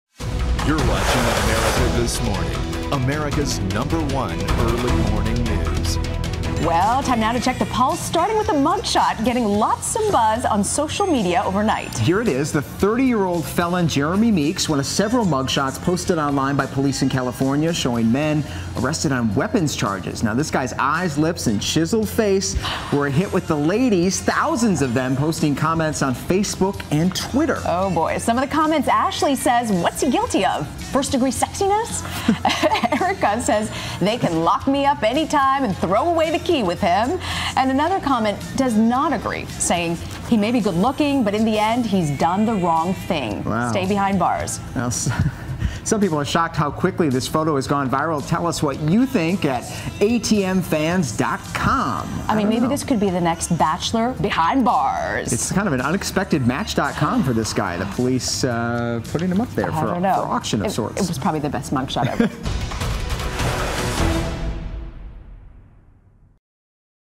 Jeremy Meeks' 'Handsome' Mug Shot Goes Viral
A California man arrested on felony weapons charges draws online praise for his looks.